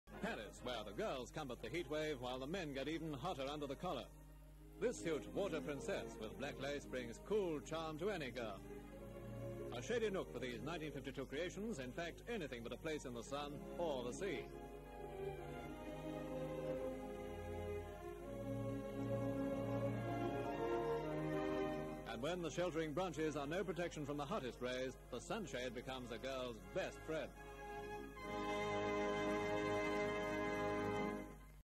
FRANCE / FASHION: Latest bathing suits modelled (1952)
Latest French bathing suit designs modelled.

Full Description:

FRANCE: Paris:
Latest designs in French swim suits to combat the heat wave
Background: Latest French bathing suit designs modelled.